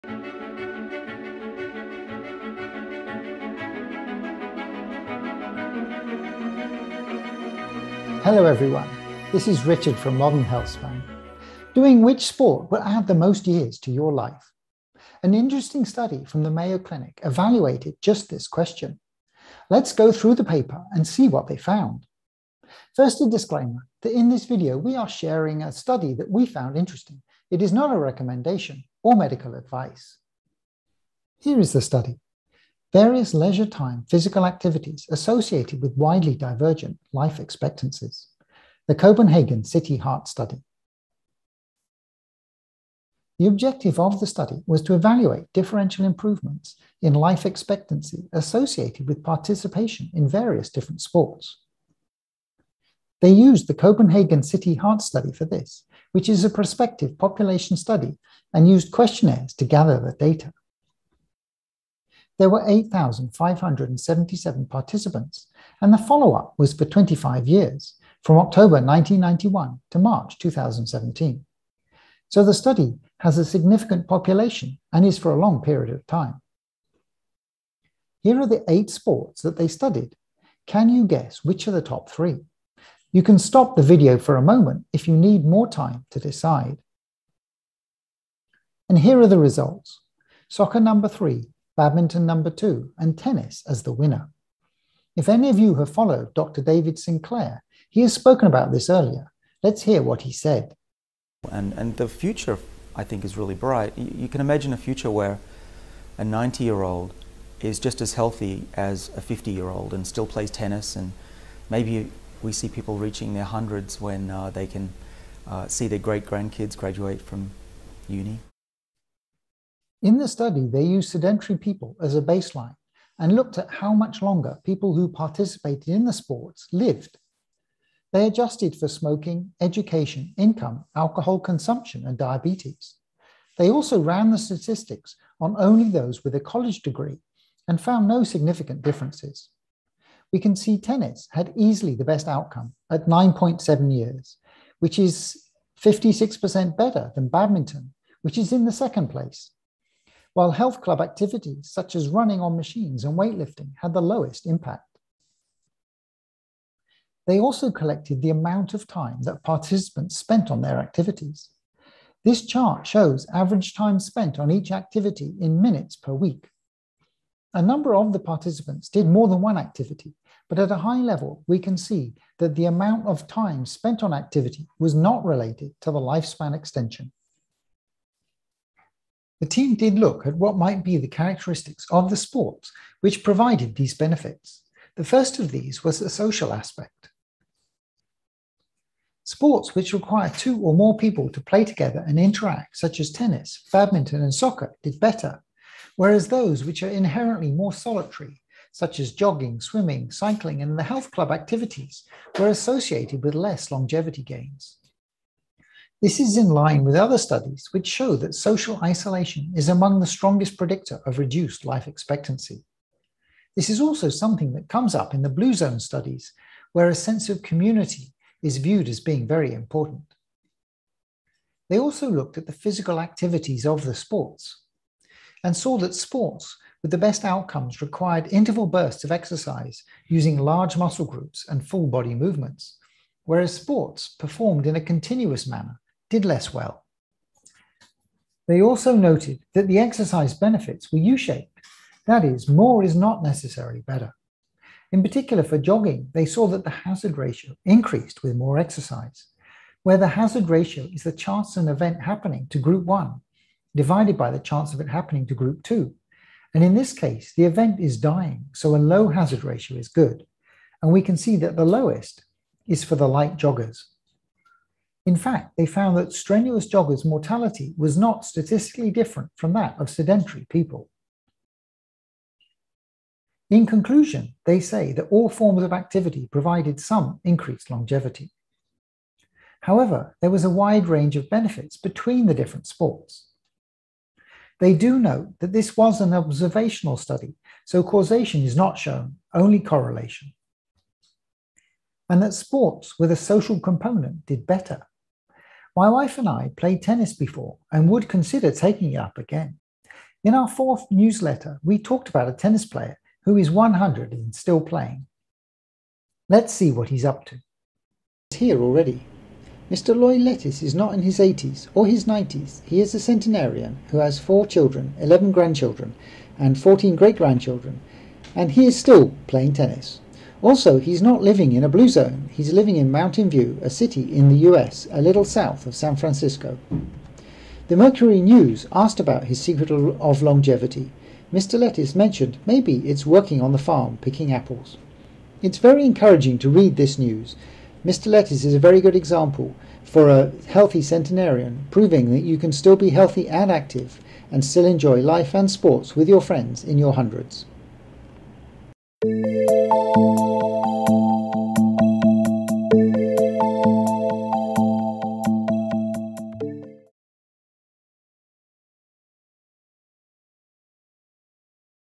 Which Sport Has The Longest Life Expectancy | Modern Healthspan
In this video we review a very interesting study that looked at various sports and how they impacted lifespan. It is well known that physical activity is good for our health and lifespan, but which is the best? And it we are to pick a sport, which one will add the most value? This study attempts to address these questions.

Papers mentioned in this video
Various Leisure-Time Physical Activities Associated With Widely Divergent Life Expectancies: The Copenhagen City Heart Study

~ New ~ Our Bone Health Supplement:
BioGaia, Osfortis with Vitamin D, 60 Capsules

(Expiration date is September 30, 2021)

10%off Link outside of North America